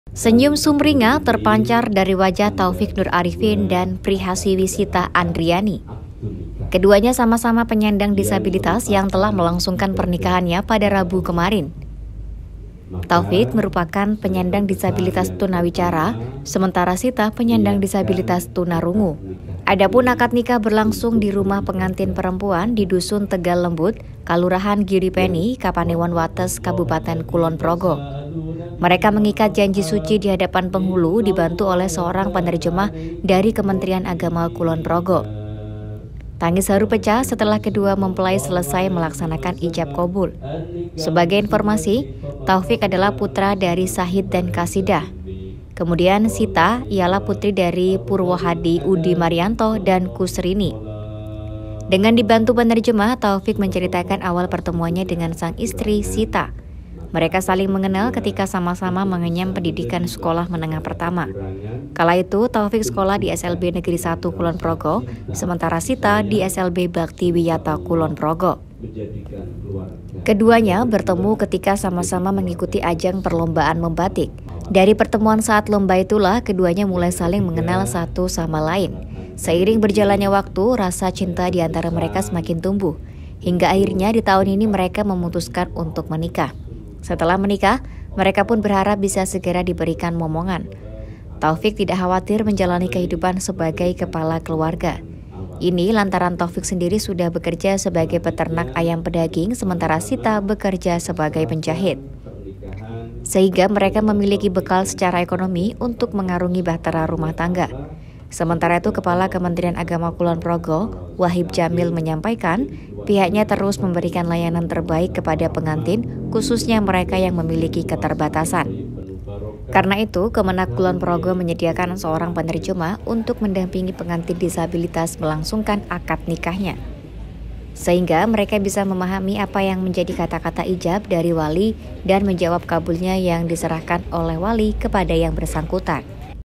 MOMEN HARU PERNIKAHAN PASANGAN DISABILITAS DI KULON PROGO
Senyum sumringah terpancar dari wajah Taufik Nur Arifin dan Prihasiwi Sita Andriyani.

Keduanya sama-sama penyandang disabilitas yang telah melangsungkan pernikahannya, pada Rabu pagi kemarin.

Taufik merupakan penyandang disabilitas tuna wicara, sementara Sita penyandang disabilitas tuna rungu.

Adapun akad nikah berlangsung di rumah pengantin perempuan di Dusun Tegal Lembut, Kalurahan Giripeni, Kapanewon Wates, Kabupaten Kulon Progo. (*)

Editor : Mona Kriesdinar
Video : Afifudin
Narator : Noristera
Produser : Ribut Raharjo
(FIF)

Sumber : Tribun Jogja